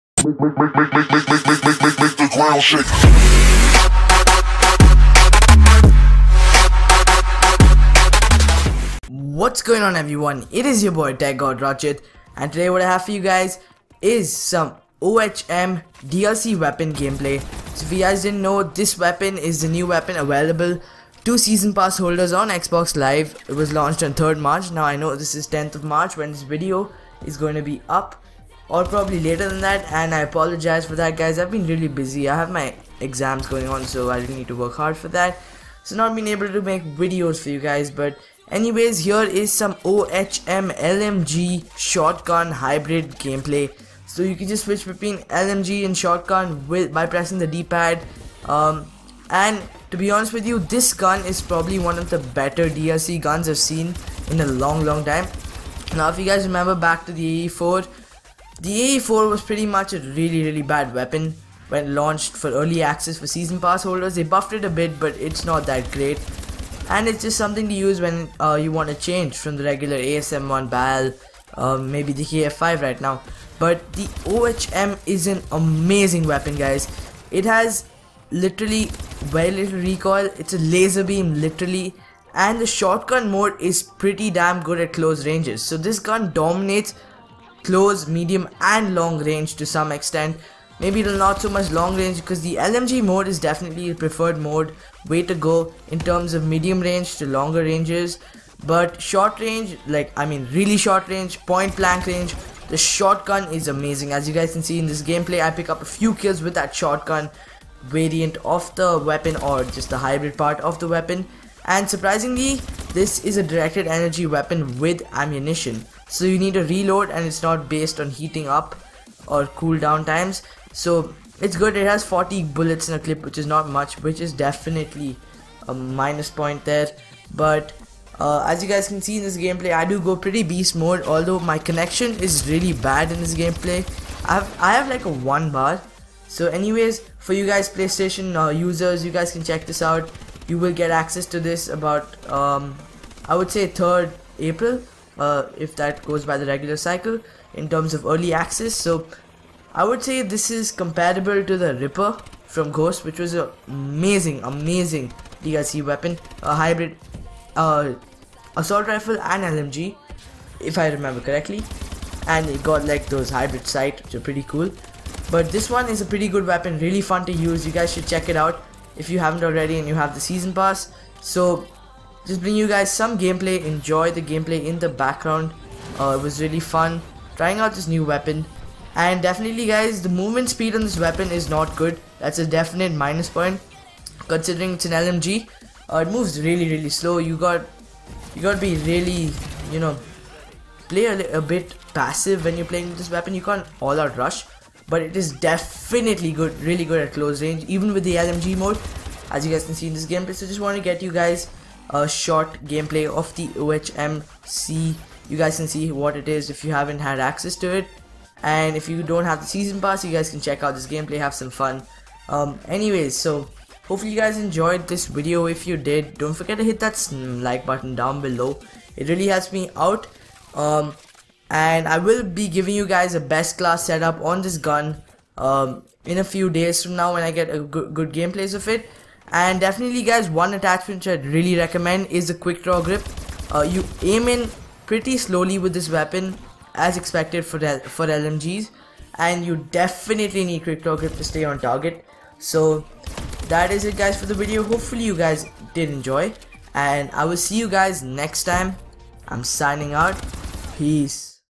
"BEST DLC WEAPON?" OHM Hybrid LMG SHOTGUN DLC Weapon | My thoughts | OHM DLC Weapon Gameplay
The NEW Ohm Hybrid Directed energy LMG and Shotgun hybrid DLC weapon was launched by Sledgehammer Last week! Here are my thoughts and First impressions! What do you guys think?